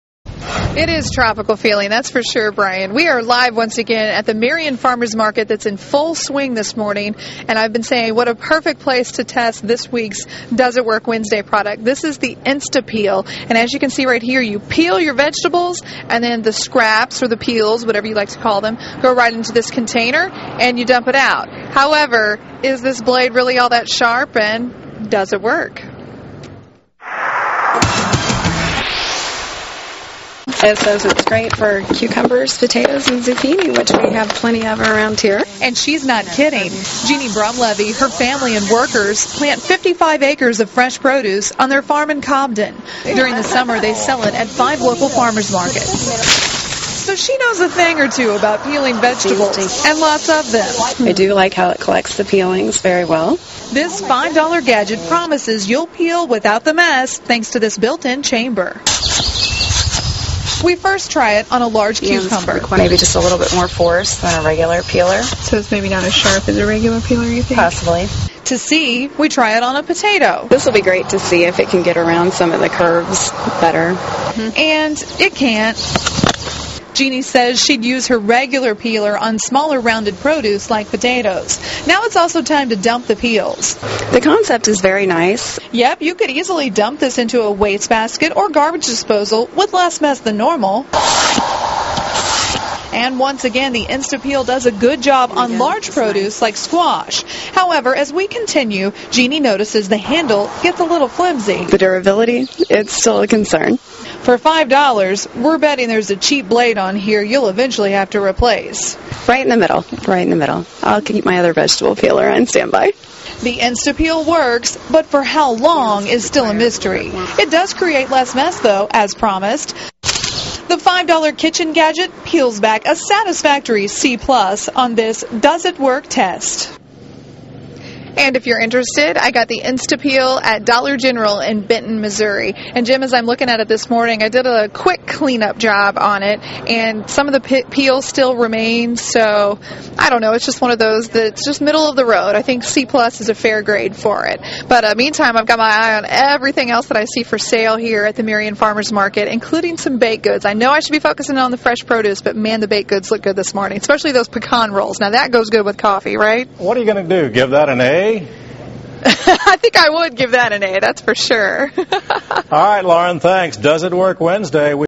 Insta Peel Does it Work?
COBDEN, IL (KFVS) - Makers of the $5 Insta Peel promise there's less mess when you peel vegetables. The peels go into a special container attached to the peeler, but Does it Work?

"It says it's great on cucumbers, potatoes and zucchini which we have a lot of around here," said Jeanne Brumleve.

She's not kidding. Jeanne Brumleve, her family and workers plant 55 acres of fresh produce on their farm in Cobden. During the summer, they sell it all at five local farmer's markets. So, she knows a thing or two about peeling vegetables...and lots of them.

"I do like how it collects peelings."

This $5 gadget promises you'll peel without the mess, thanks to this built-in chamber. We first try it on a large cucumber.

"You have to use just a little more force than a regular peeler."

To test that, we try it on a potato.

"This will be great to see if it can get around some of the curves better."

And...it can't. Jeanne says she'd use her regular peeler on smaller, rounded produce like potatoes. Now it's also time to dump the peels.

"The concept is nice."

You could easily dump the peels into a wastebasket or garbage disposal with less mess than normal. Once again, the Insta Peel does a good job on large produce like squash. However, as we continue, Jeanne notices the handle gets a little flimsy.

"The durability is still a concern."

For $5, we're betting there's a cheap blade you'll eventually have to replace.

"I'd give it a grade right in the middle. I'll keep my other peeler on standby."

The Insta Peel works, but for how long is still a mystery. It does create less mess, as promised. The $5 kitchen gadget peels back a satisfactory C+ on this Does it Work test.  Lauren bought Insta Peel at Dollar General in Benton, Missouri.